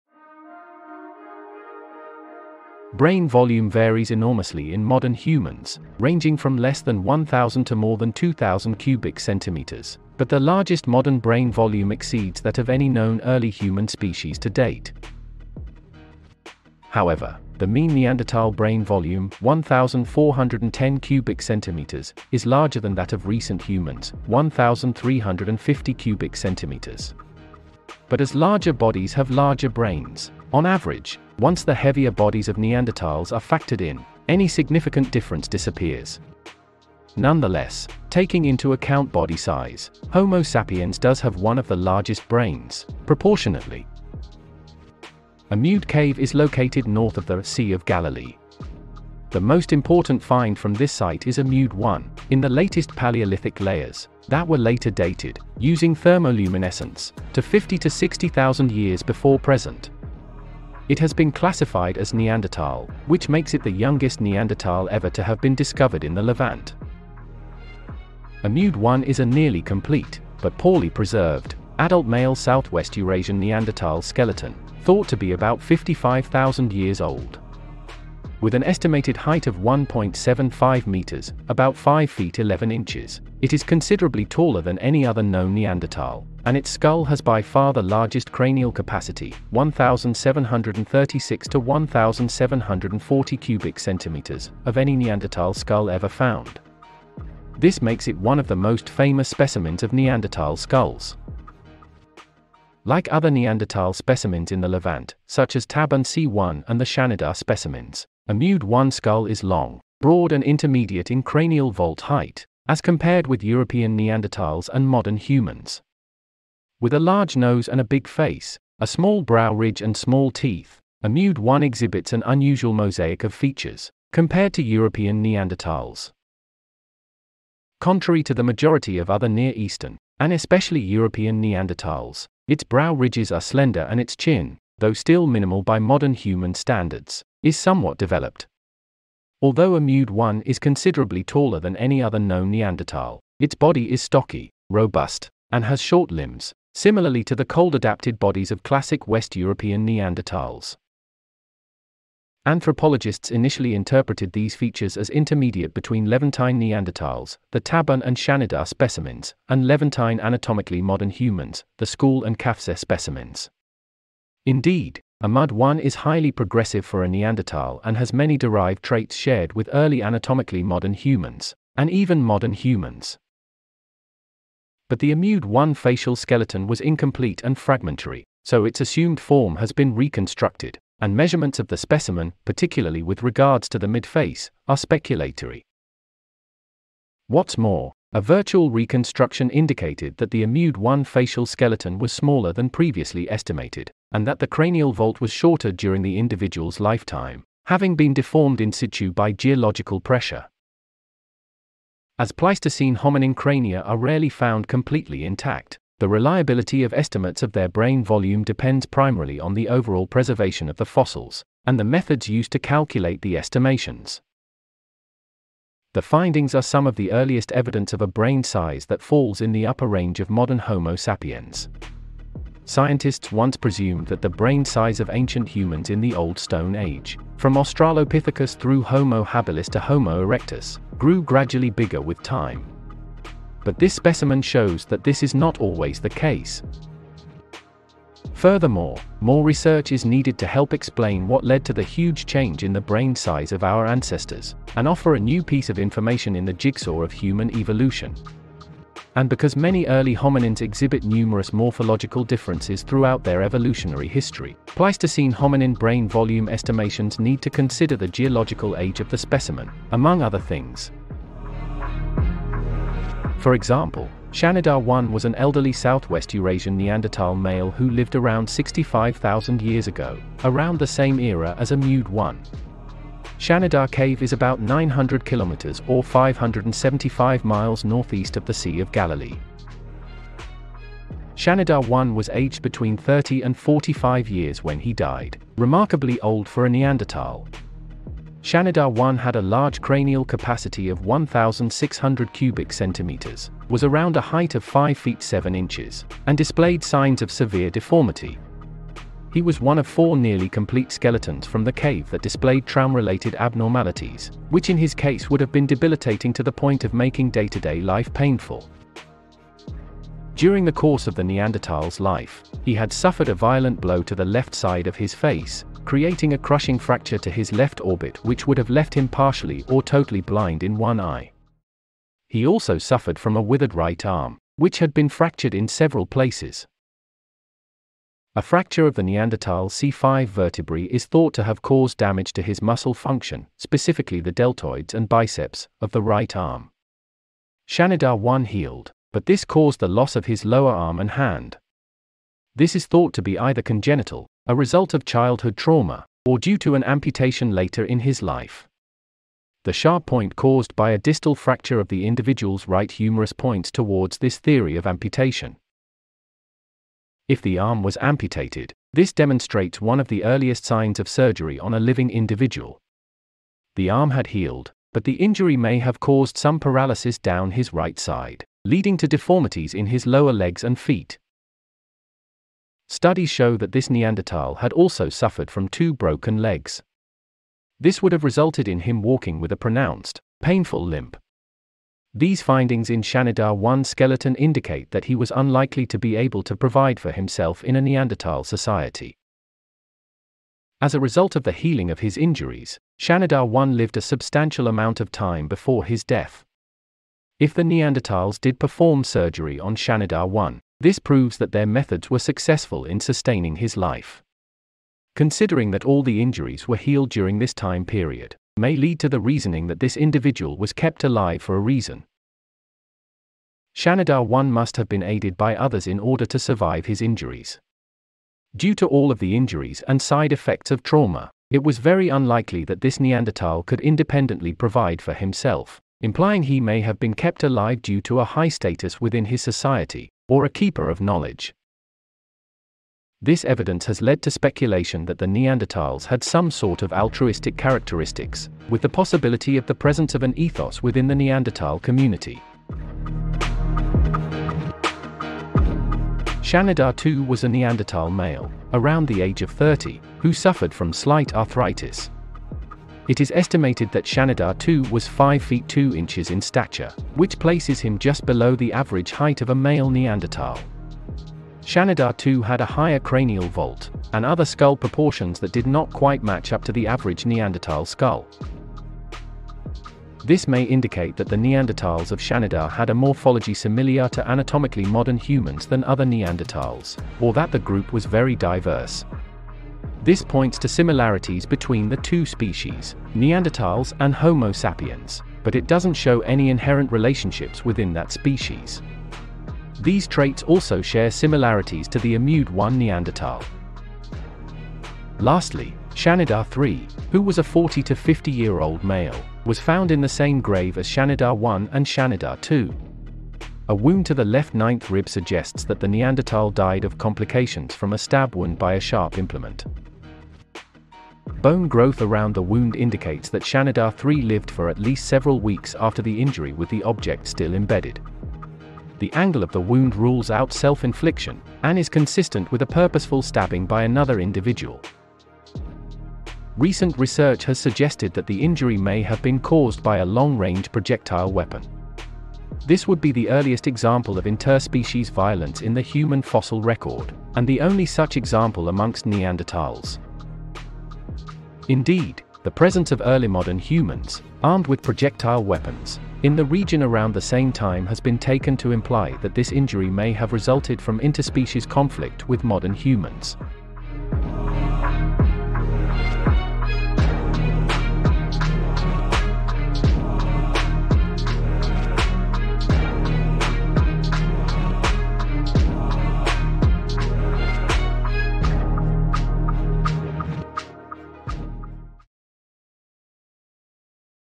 Incredible discovery sheds new light on the Brain Size of Neanderthals
In this video, archaeologists have discovered the biggest Neanderthal brain yet!  This is an incredible discovery that sheds new light on the cognitive abilities of Neanderthals. The find suggests that Neanderthals were smarter than previously thought, and that they may have been able to think abstractly.

If you're interested in archaeology, be sure to watch this video to learn more about the biggest Neanderthal brain ever discovered!

Brain volume varies enormously in modern humans, ranging from less than 1,000 to more than 2,000 cm3, but the largest modern brain volume exceeds that of any known early human species to date.  However, the mean Neanderthal brain volume (1,410 cm3 ) is larger than that of recent humans (1,350 cm3).  But as larger bodies have larger brains, on average, once the heavier bodies of Neanderthals are factored in, any significant difference disappears.  Nonetheless, Taking into account body size, Homo sapiens does have one of the largest brains, proportionately.